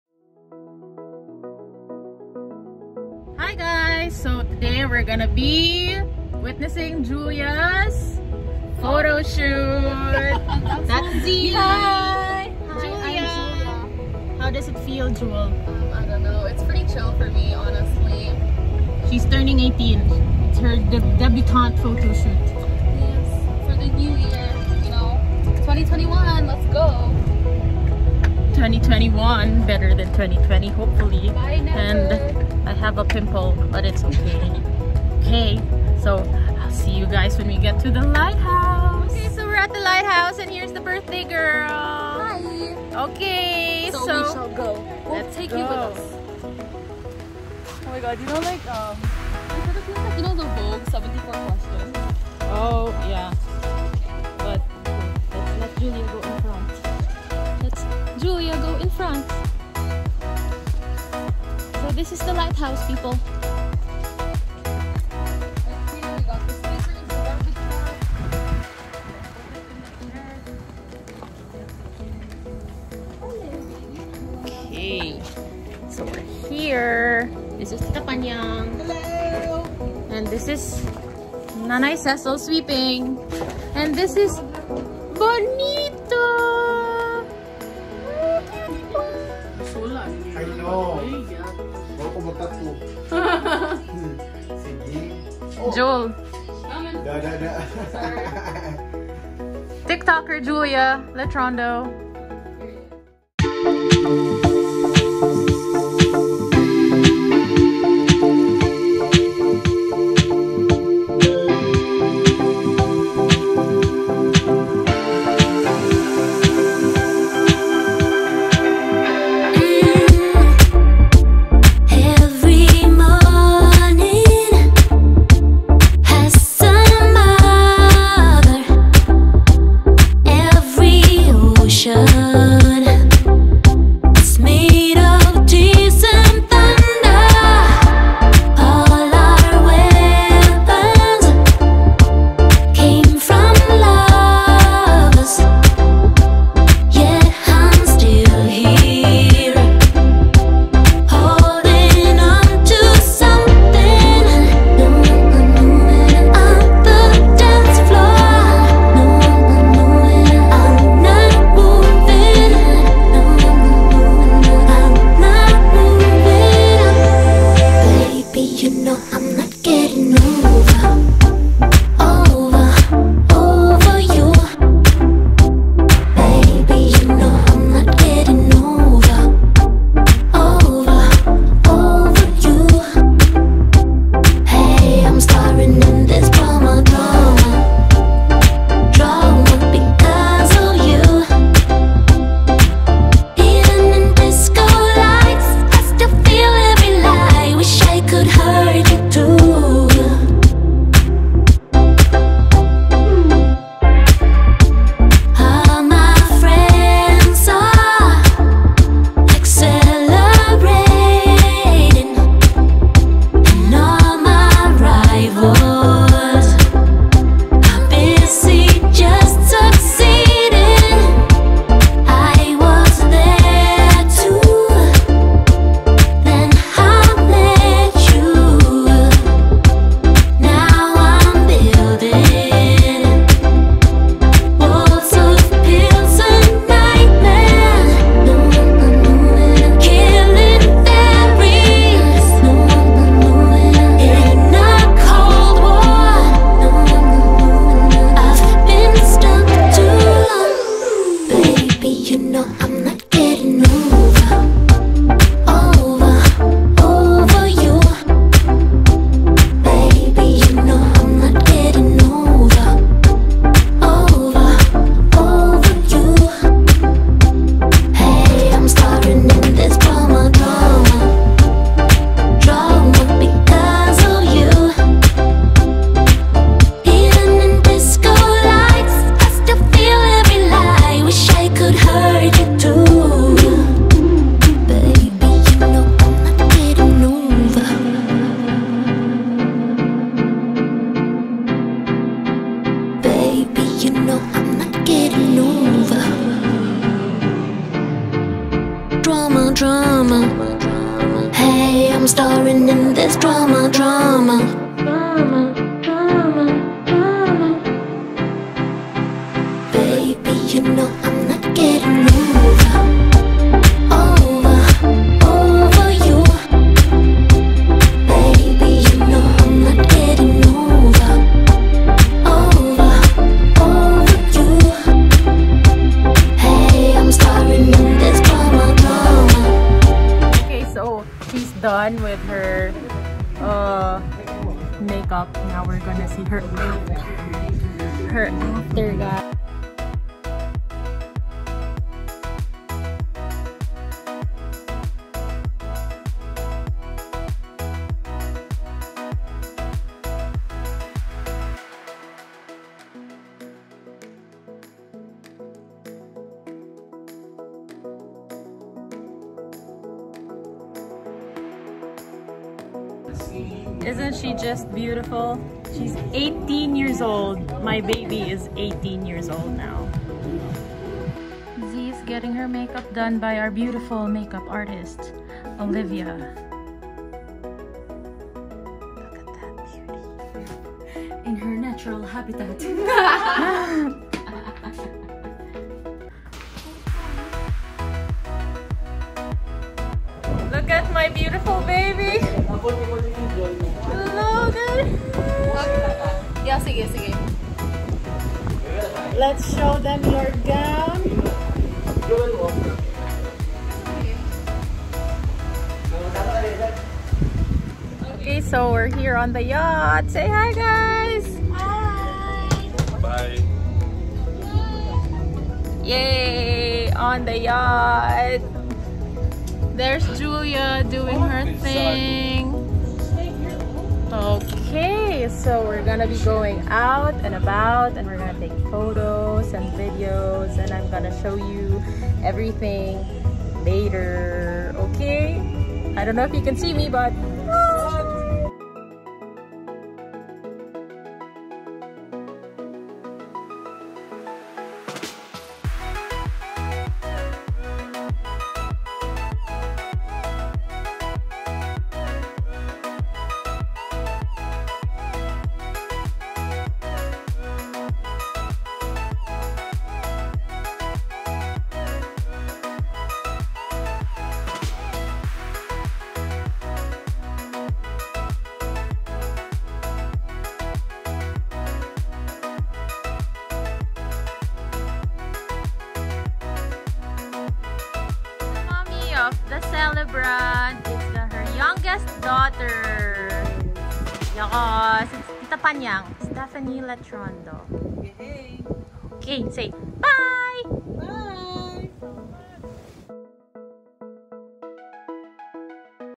My Cousins Pre-Debut Shoot- BTS
My cousin Julia just turned 18 and I took the liberty to shoot some behind the scenes videos and shots of her, the venue and my family! Hope you guys enjoy this short watch :)